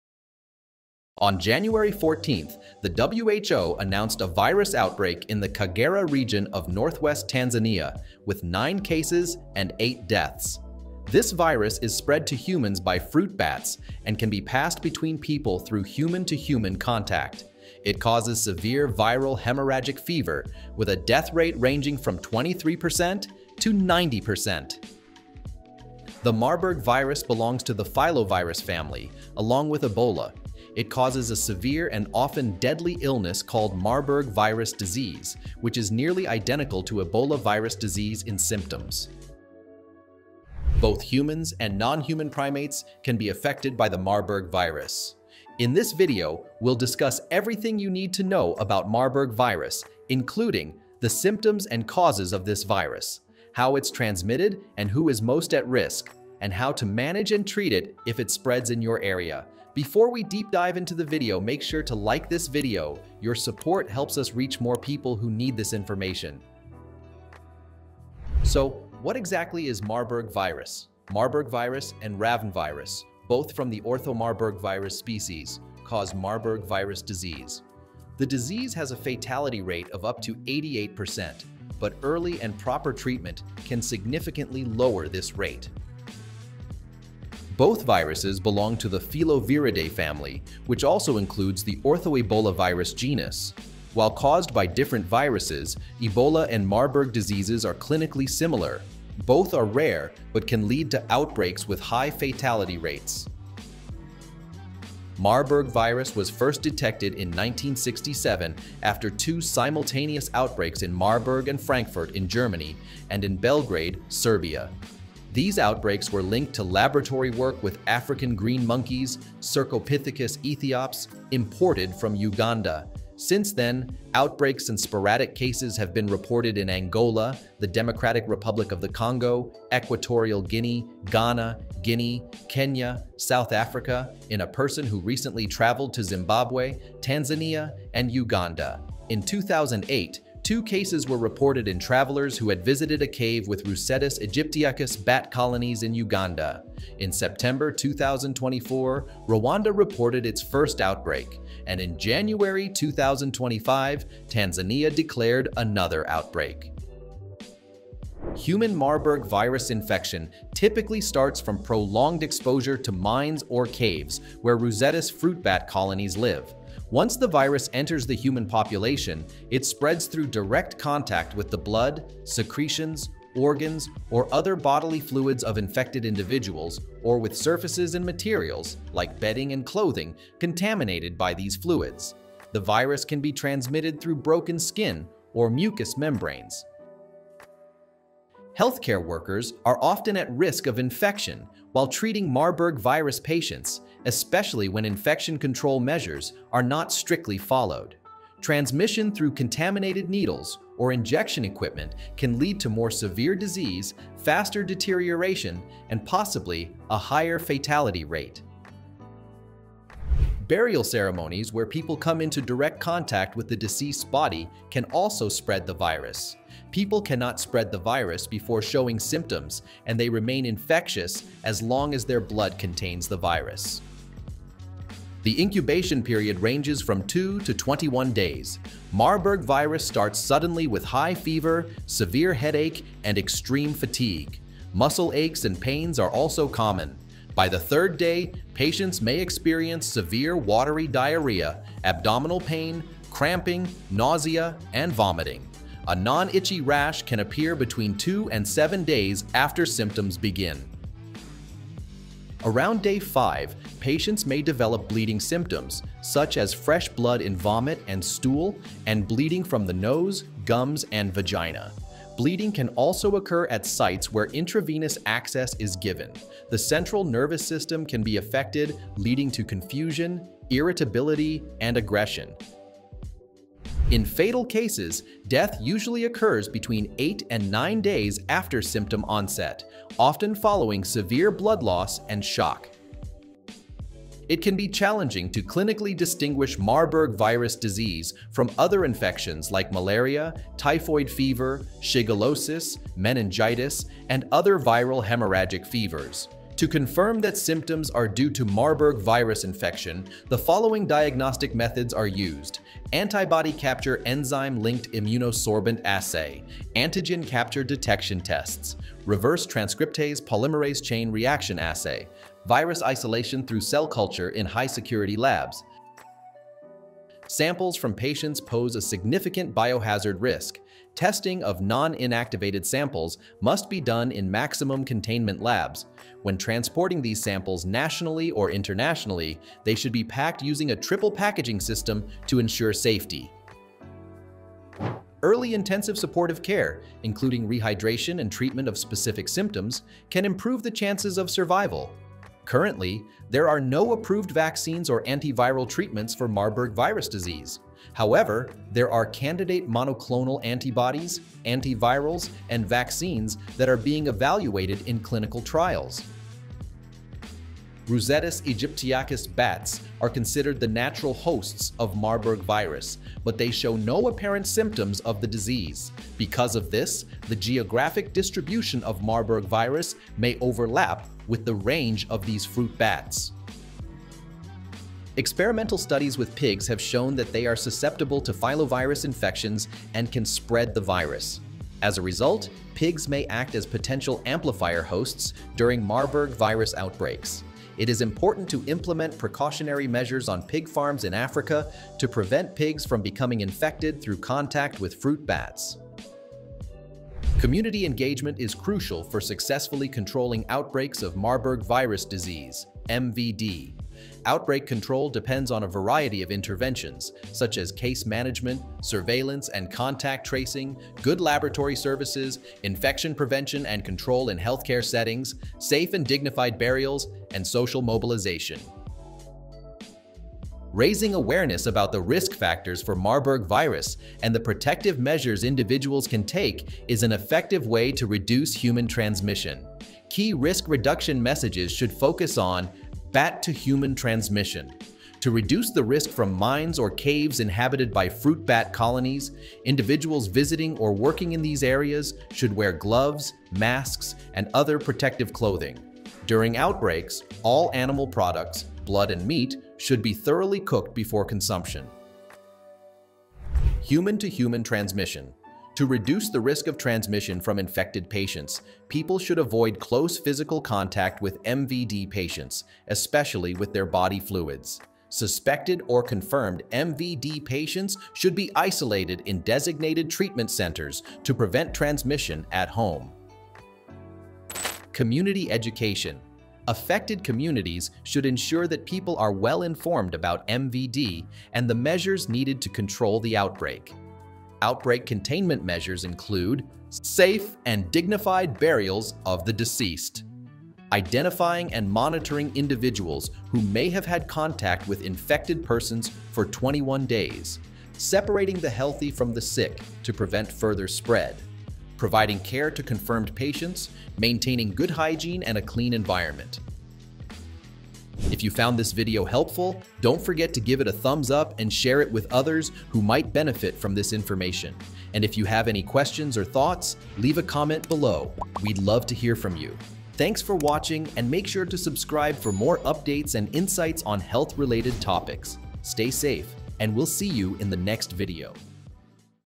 Marburg virus: The Rapidly Spreading Virus You Haven’t Heard Of
The Marburg virus is causing alarm as outbreaks emerge, posing serious health risks, especially in vulnerable communities. In this video, we break down everything you need to know about this deadly virus:

✅ What is the Marburg virus and why is it dangerous?
✅ Symptoms and warning signs to look out for
✅ How it spreads and who is most at risk
✅ Critical prevention and management strategies

With no approved vaccine or specific treatment, awareness and early action are crucial in preventing the spread.